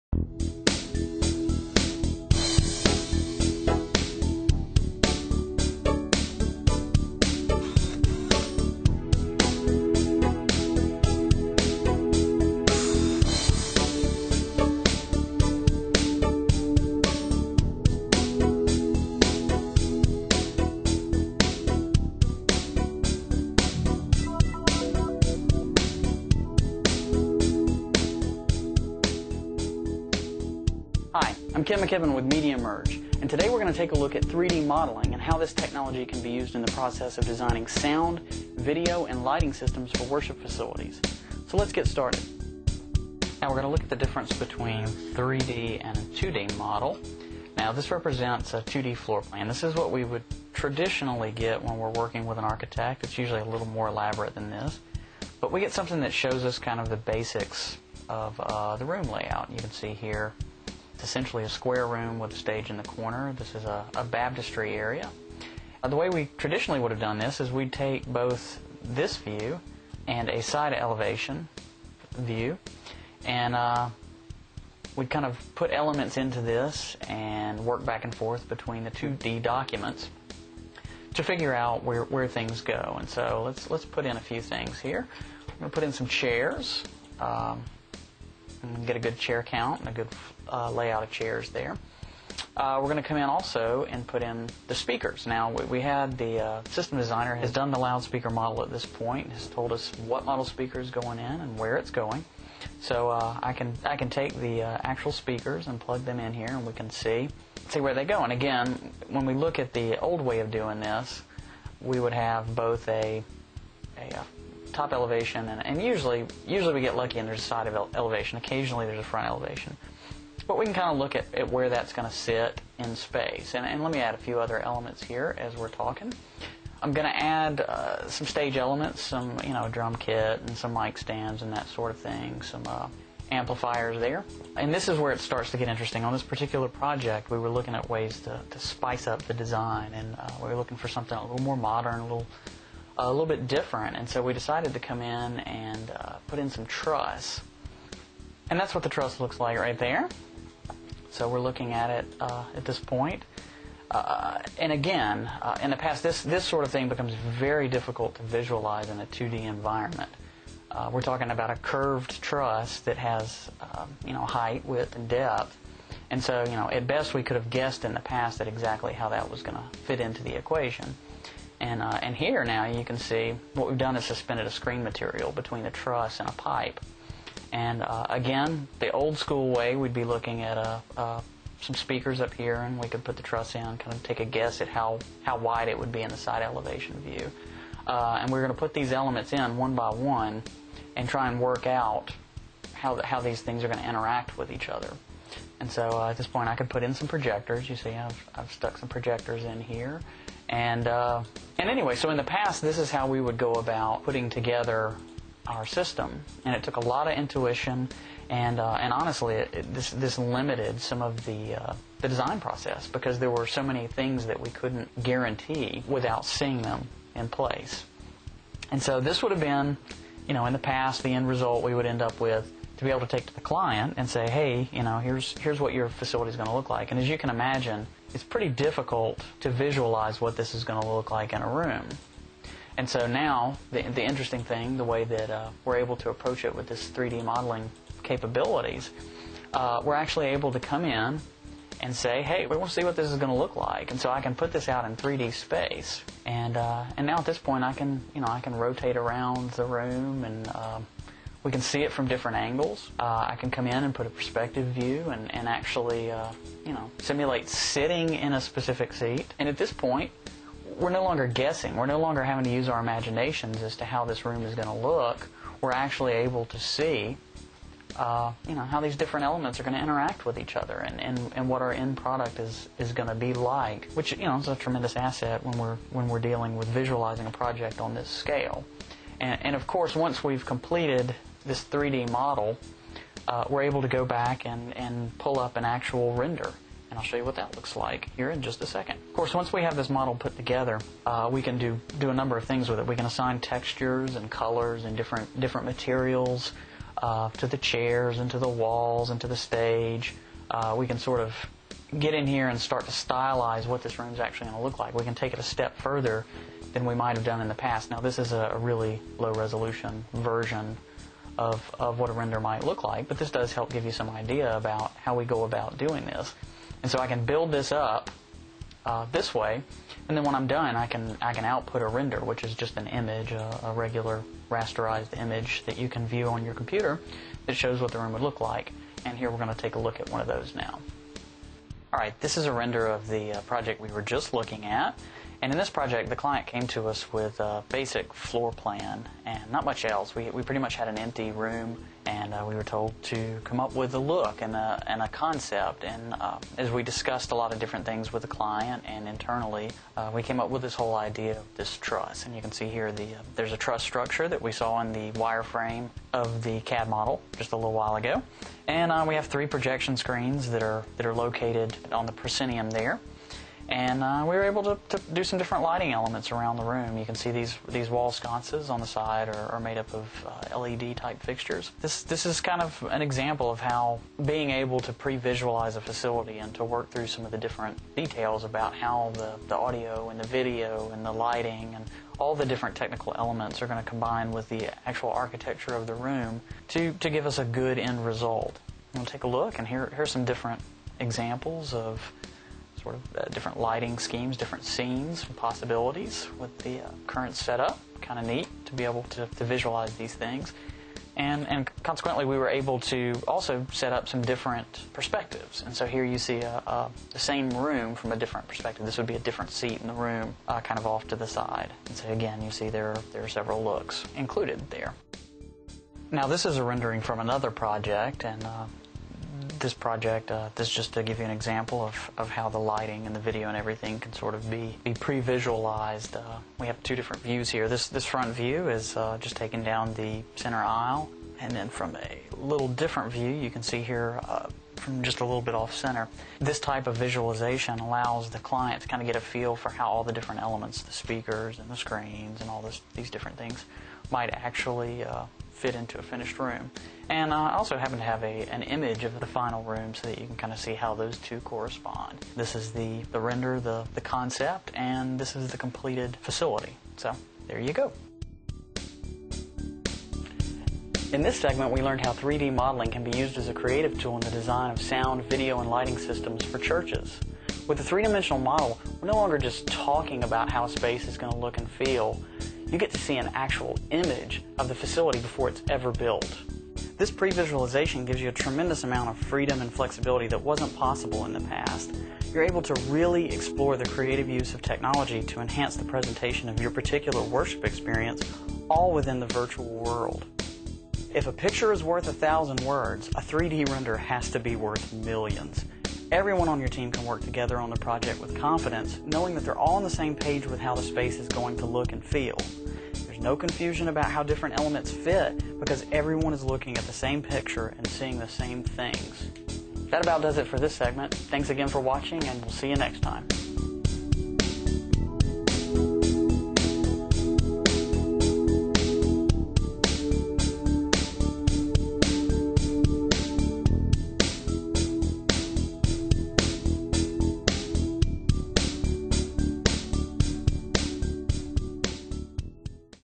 3D computer modeling for church sound, video and lighting
Predictive Modeling is the first tutorial in the Inspired Servant video series. In this video, Ken McKibben of MediaMerge demonstrates how computer-based 3D modeling can be used as part of the design process to keep everyone on the same page while exploring all of the creative possibilities for your space.

The traditional 2D approach requires you to look at floorplan and elevation drawings on paper and somehow decipher how a space is going to look and feel. This is very difficult for most non-technical committee members.

The new 3D approach delivers a photo-realistic rendering that is, in essence, a picture of the final project, complete with textures and colors as they will look in real life. Instead of the entire group coming to separate subjective conclusions on how your new facility is going to look based on drawings, the whole team can view the same image and instantly be on the same page with one another.